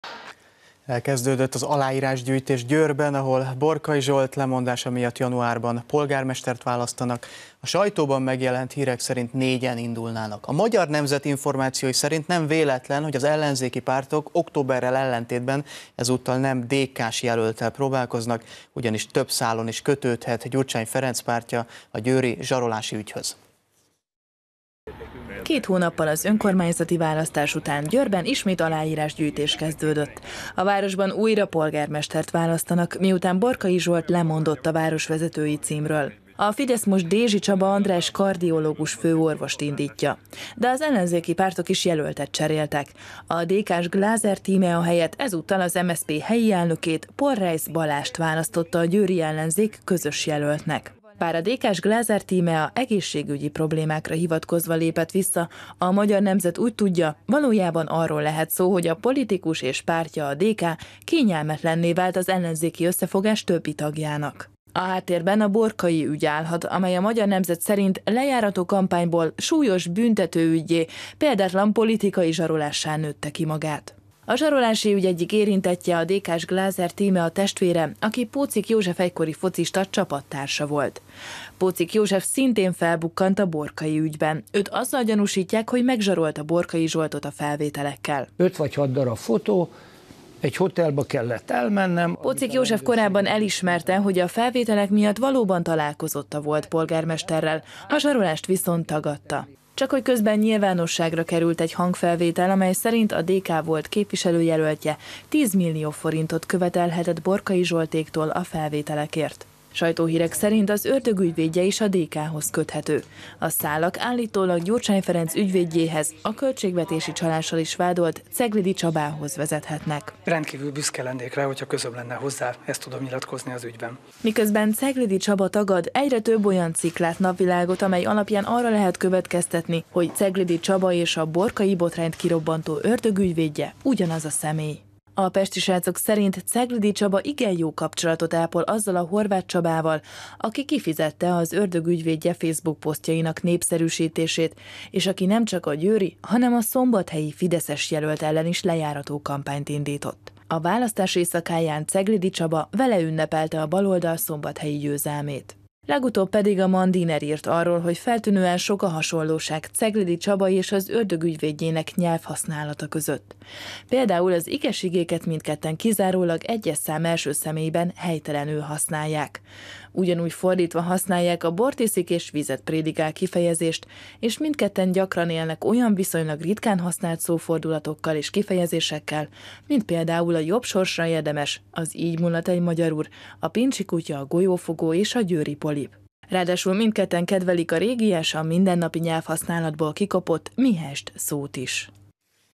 Magyar Nemzet: ráégett a DK-ra a győri zsarolási ügy
Elkezdődött az aláírásgyűjtés Győrben, ahol Borkai Zsolt lemondása miatt január végén polgármestert választanak. A sajtóban megjelent hírek szerint négyen indulnának. A Magyar Nemzet információi szerint nem véletlen, hogy az ellenzéki pártok - októberrel ellentétben - most nem DK-s jelölttel próbálkoznak, ugyanis több szálon is kötődhet Gyurcsány Ferenc pártja a győri zsarolási ügyhöz.